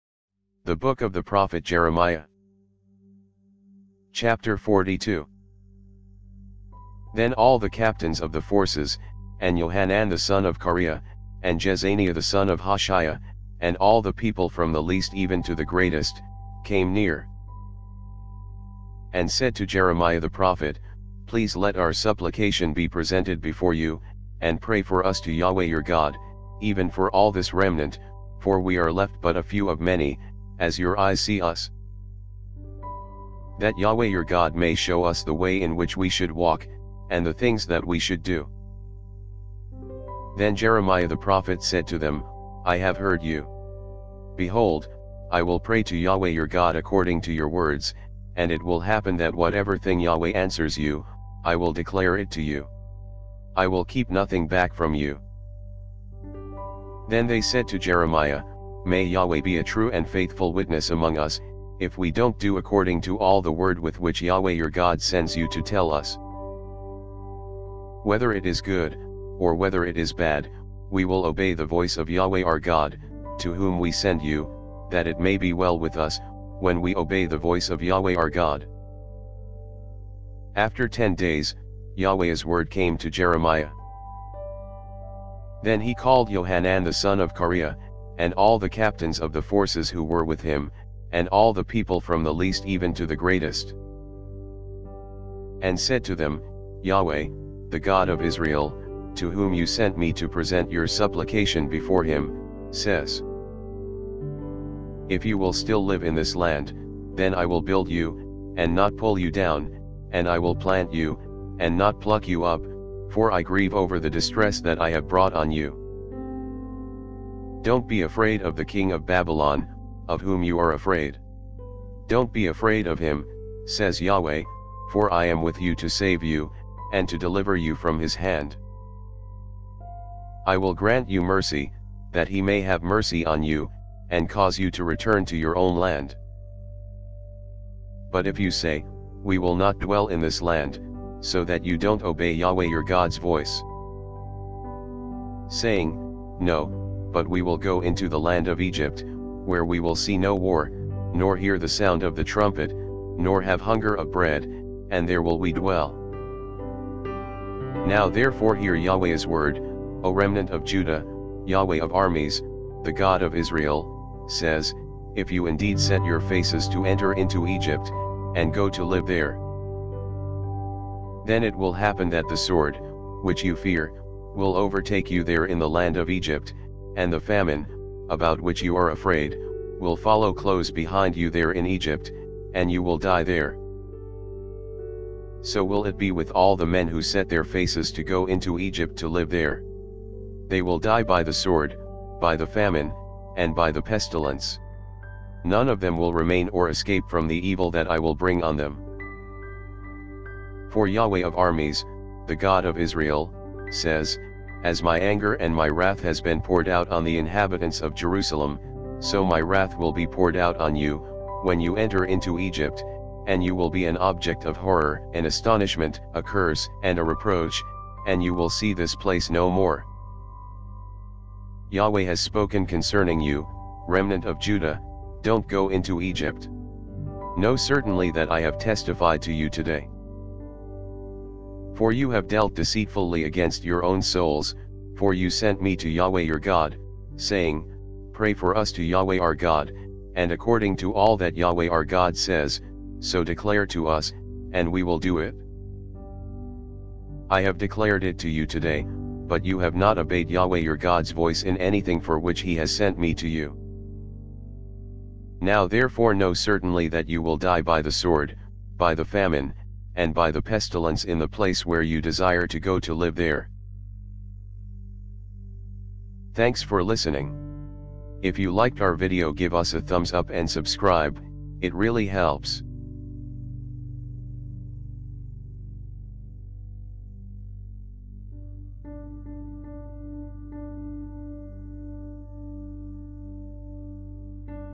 The Book of the Prophet Jeremiah - 42
The Book of the Prophet Jeremiah - Chapter 42
Source: World English Bible (public domain)

This video is a read aloud audio book of the world english bible

Art: "Jeremiah Lamenting the Destruction of Jerusalem" Rembrandt van Rijn (public domain)